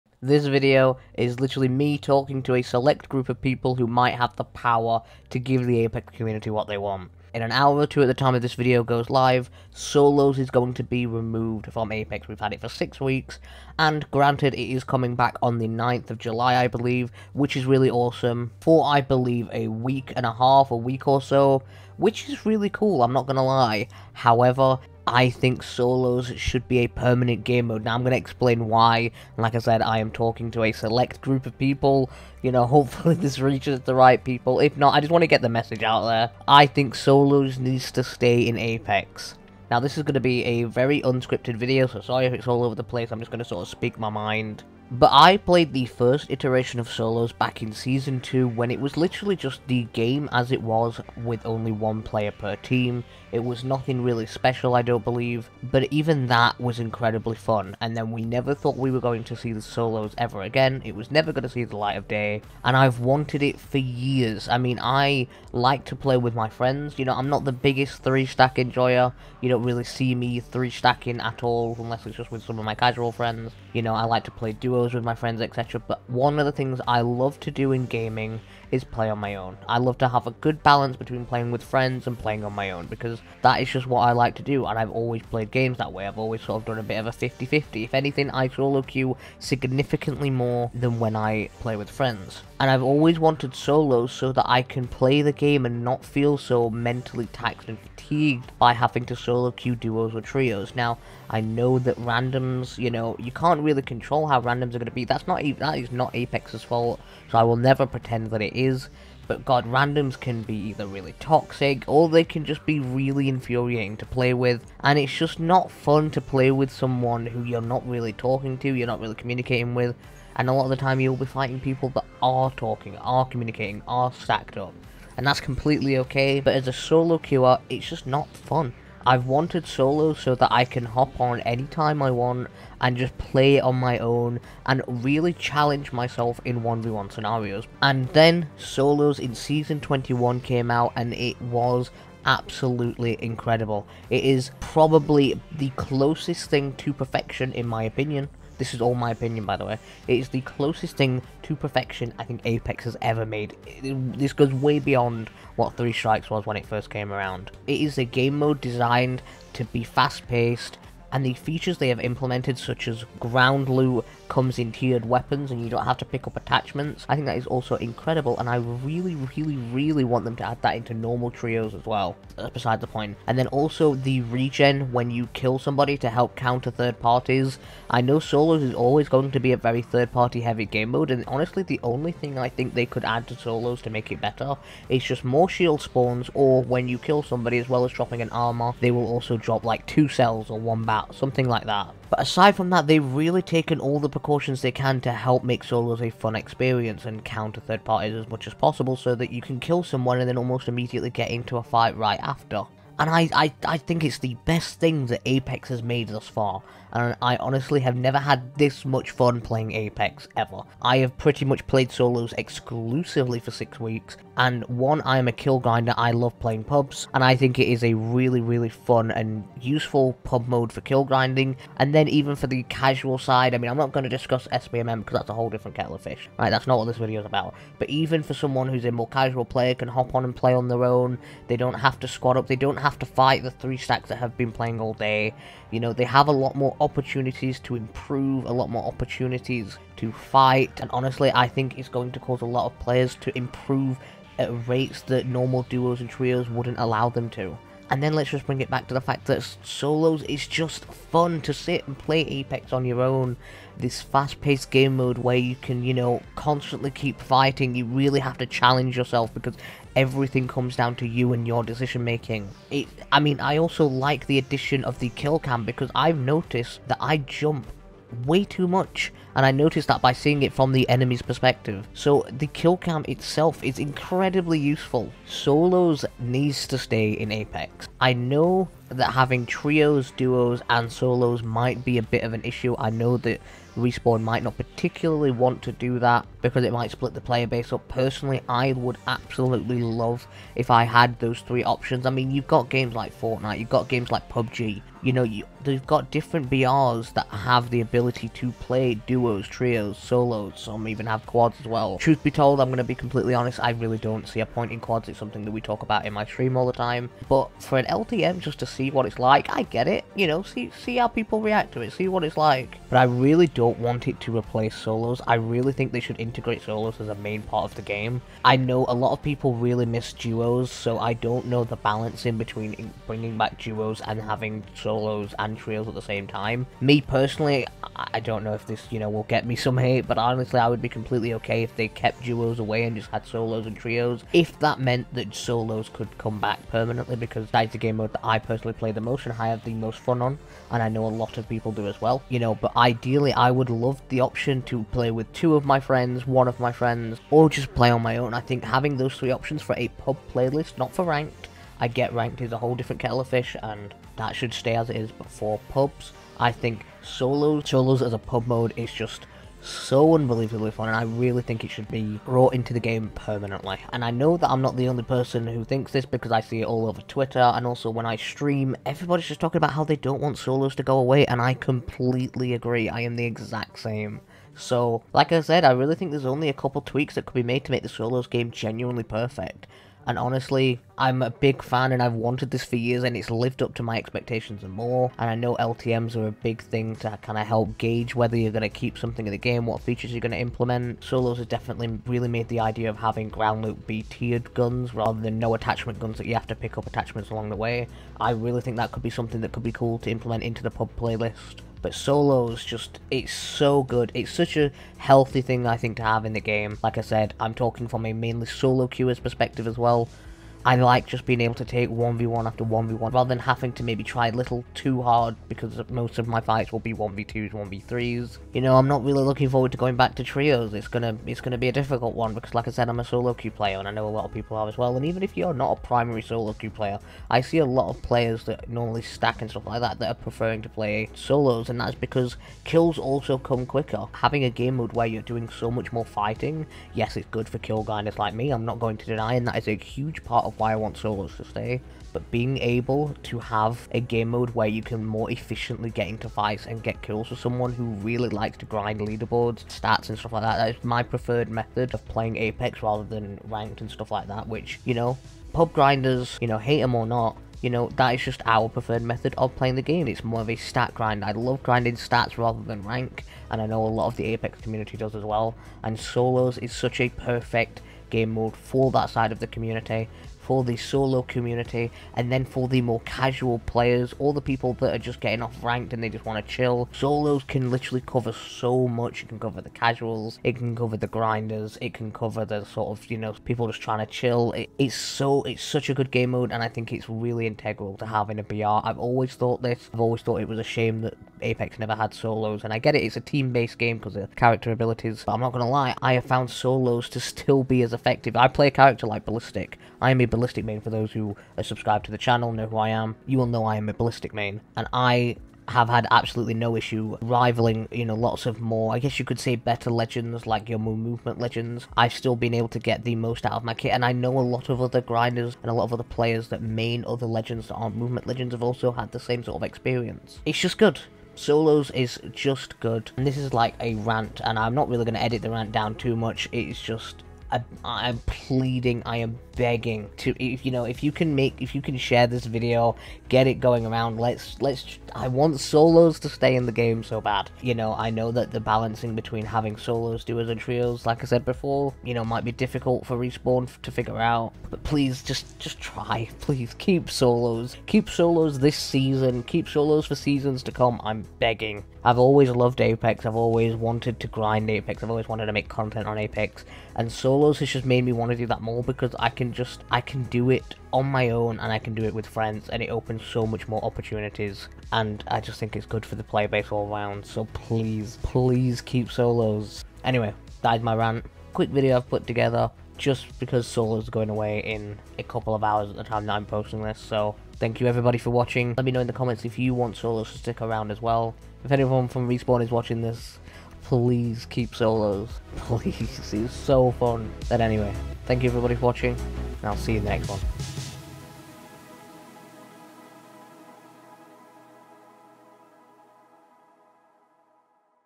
Respawn, please keep solos...
Explaining why solos should stay in Apex Legends